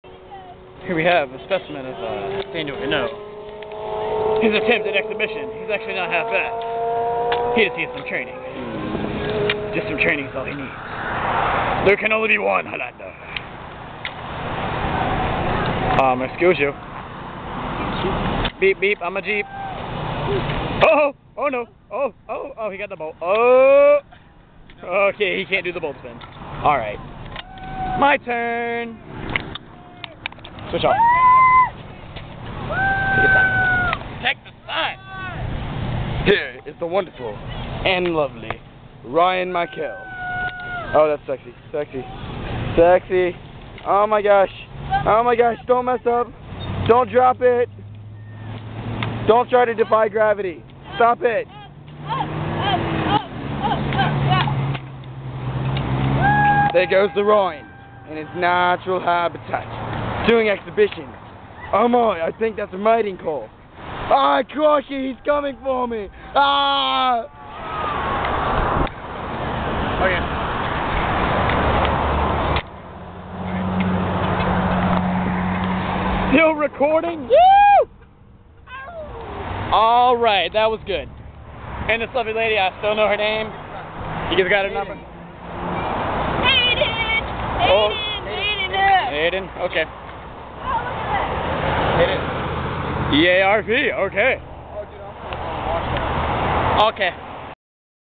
Exhibition and Hayden saying "HAYDEN, HAYDENNN, HAYDEN!" to the camera.
Bored, trying to attract people to the car wash with exhibition. :P